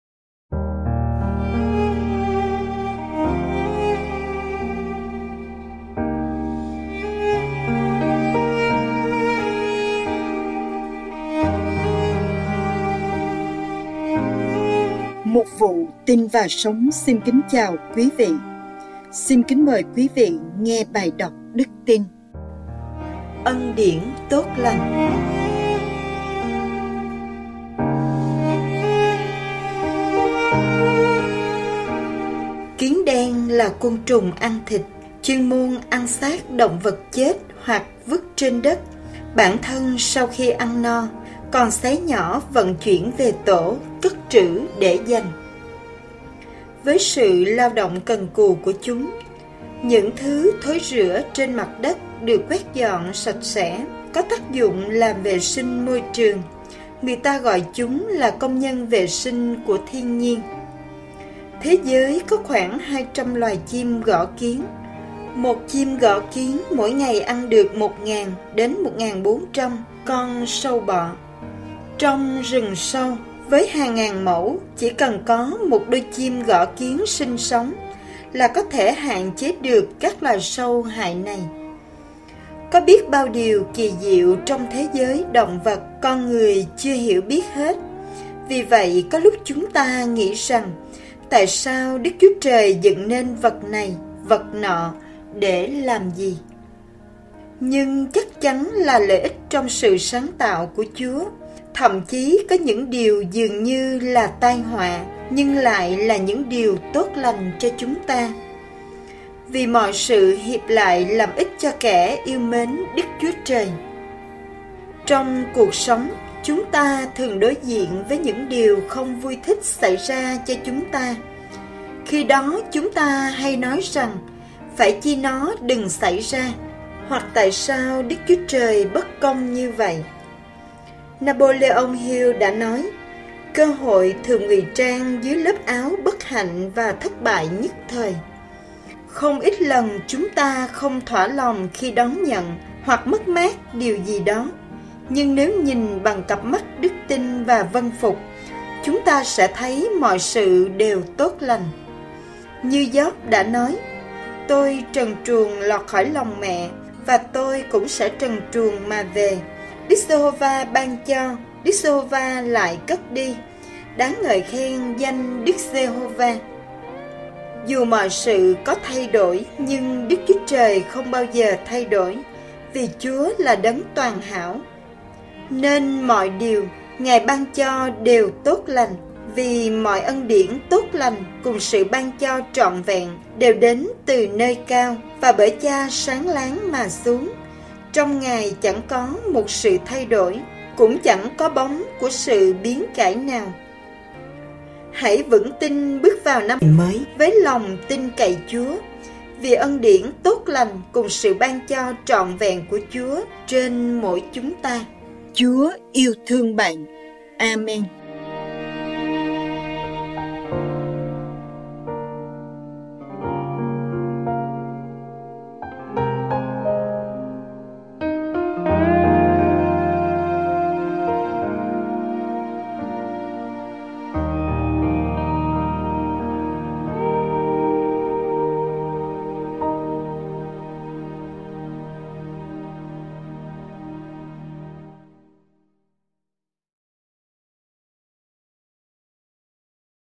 Ân Điển Tốt Lành
Kiến đen là côn trùng ăn thịt, chuyên môn ăn xác động vật chết hoặc vứt trên đất. Bản thân sau khi ăn no, còn xé nhỏ vận chuyển về tổ tích trử, để dành. Với sự lao động cần cù của chúng, những thứ thối rữa trên mặt đất được quét dọn sạch sẽ, có tác dụng làm vệ sinh môi trường. Người ta gọi chúng là công nhân vệ sinh của thiên nhiên.
Thế giới có khoảng 200 loài chim gõ kiến. Một con chim gõ kiến mỗi ngày ăn được 1000 đến 1400 con sâu bọ. Trong rừng sâu, với hàng ngàn mẫu, chỉ cần có một đôi chim gõ kiến sinh sống là có thể hạn chế được các loài sâu hại này. Có biết bao điều kỳ diệu trong thế giới động vật con người chưa hiểu biết hết. Vì vậy có lúc chúng ta nghĩ rằng, “Tại sao Đức Chúa Trời dựng nên vật này, vật nọ để làm gì?” Nhưng kích trắng là lợi ích trong sự sáng tạo của Chúa, thậm chí có những điều dường như là tai họa, nhưng lại là những điều tốt lành cho chúng ta. “Vì một sự hiệp lại làm ích cho kẻ yêu mến Đức Chúa Trời.” (Rô-ma 8:28)
Trong cuộc sống chúng ta thường đối diện với những điều không vui thích xảy ra cho chúng ta. Khi đó chúng ta hay nói rằng, “Phải chi nó đừng xảy ra!” hoặc, “Tại sao Đức Chúa Trời bất công như vậy?” Napoleon Hill đã nói, “Cơ hội thường ngụy trang dưới lớp áo bất hạnh và thất bại nhất thời.” Không ít lần chúng ta không thỏa lòng khi đón nhận, hoặc mất mát điều gì đó. Nhưng nếu nhìn bằng cặp mắt đức tin và vâng phục, chúng ta sẽ thấy mọi sự đều tốt lành. Như Gióp đã nói, “Tôi trần truồng lọt khỏi lòng mẹ, Và tôi cũng sẽ trần truồng mà về; Đức Giê-hô-va ban cho, Đức Giê-hô-va lại cất đi. Đáng ngợi khen danh Đức Giê-hô-va.” (Gióp 1:21)
Dù mọi sự có thay đổi, nhưng Đức Chúa Trời không bao giờ thay đổi. Vì Chúa là Đấng toàn hảo, nên mọi điều Ngài ban cho đều tốt lành. “Vì mọi ân điển tốt lành, cùng sự ban cho trọn vẹn đều đến từ nơi cao và bởi Cha sáng láng mà xuống. Trong Ngài chẳng có một sự thay đổi, cũng chẳng có bóng của sự biến cải nào.” (Gia-cơ 1:27)
Hãy vững tin bước vào năm mới với lòng tin cậy Chúa, vì ân điển tốt lành cùng sự ban cho trọn vẹn của Chúa trên mỗi chúng ta.
Chúa yêu thương Bạn! Amen!